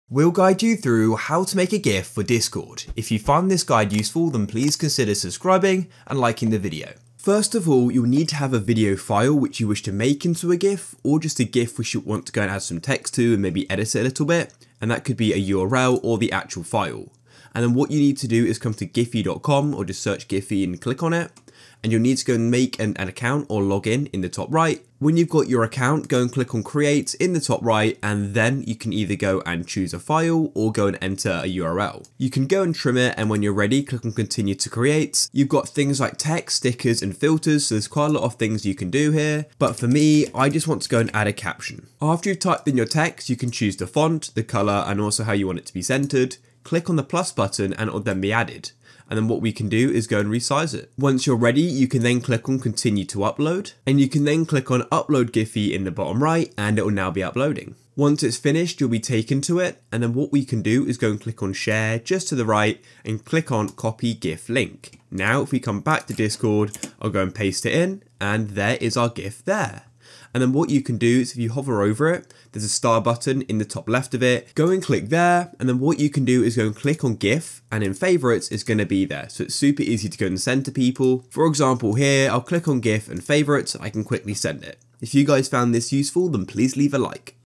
How To Make A GIF For Discord - Full Guide
Learn how to make a gif for discord in this video.

GuideRealm